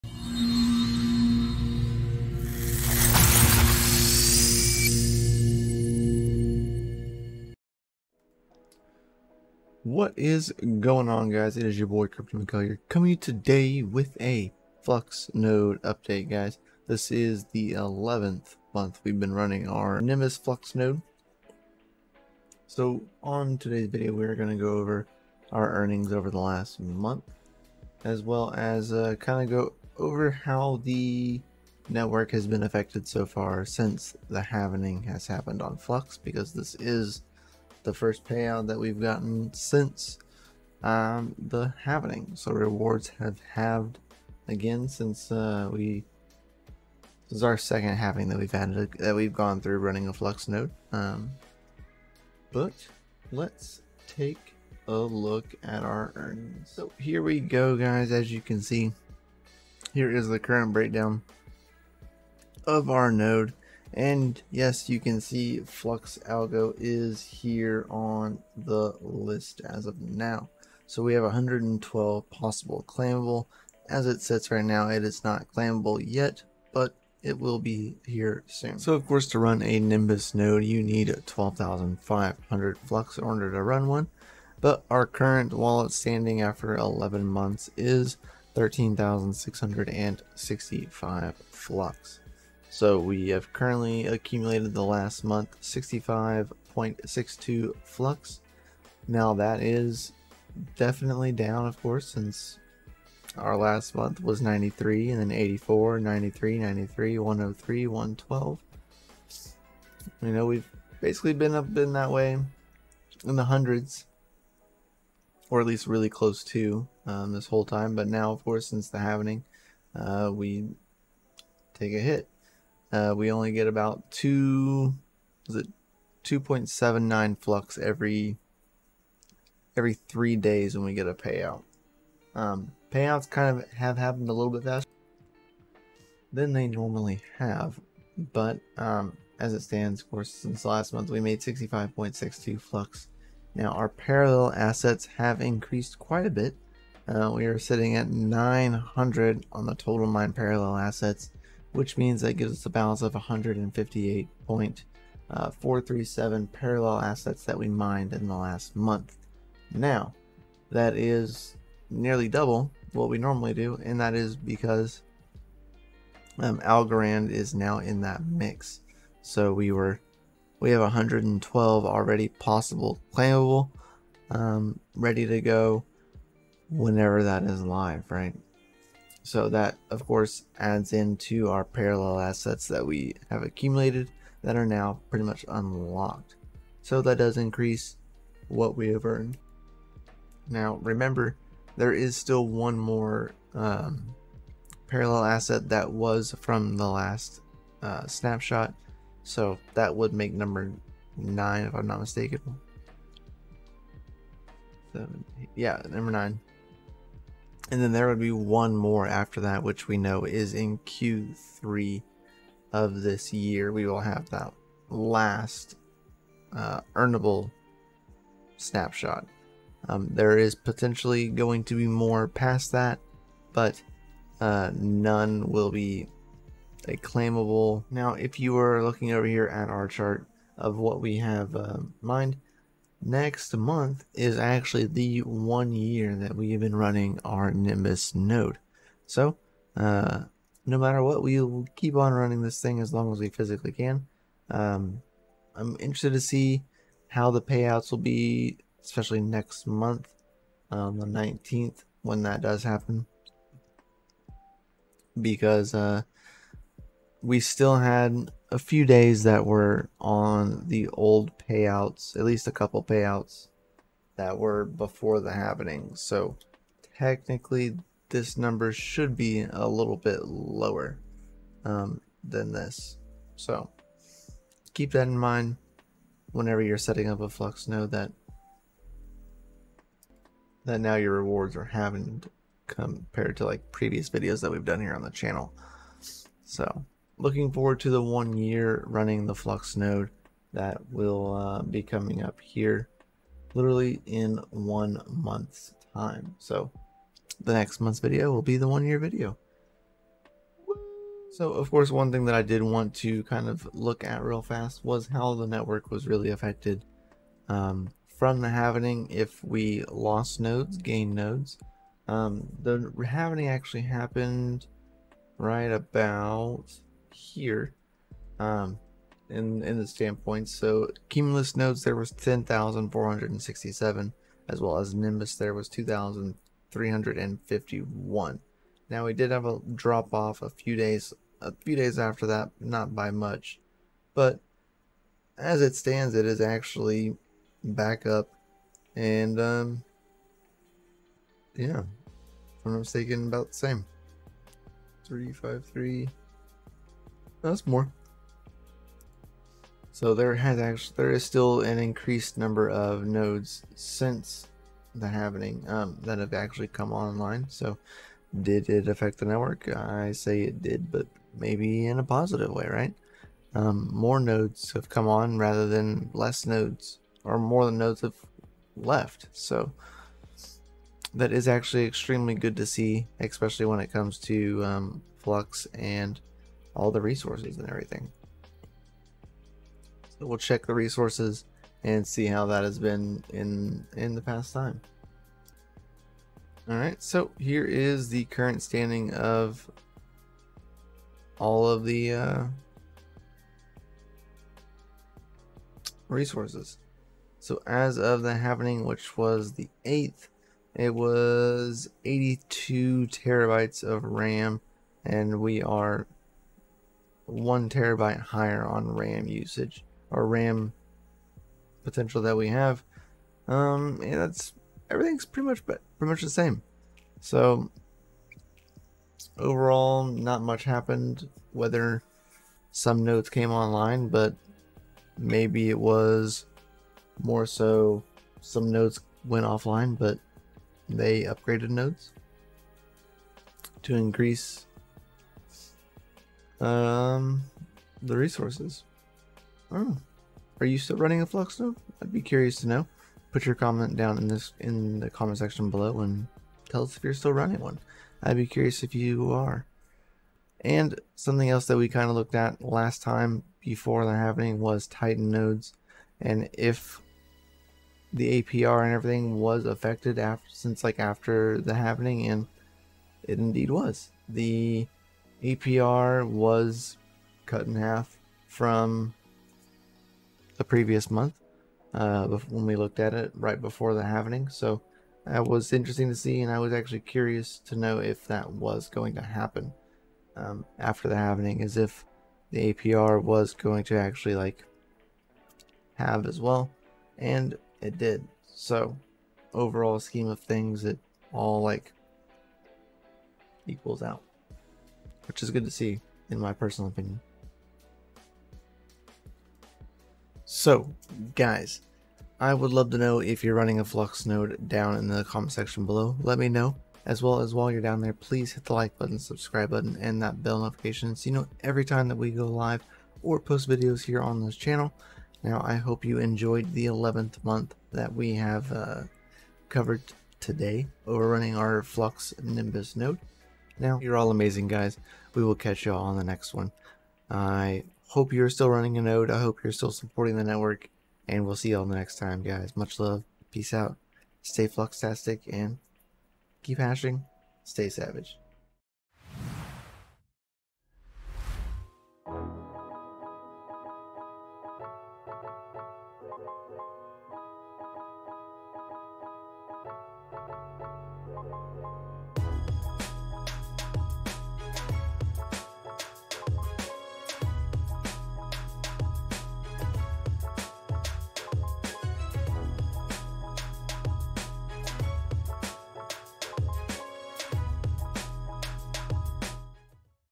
Node Update - 11th Month Nimbus Node Earnings And There Are More Nodes Online After Halving?
Today we will be going over the 11th months earnings from running our Nimbus FluxNode. We will also go over if there are more node online since the halving and the answer JUST might shock you!!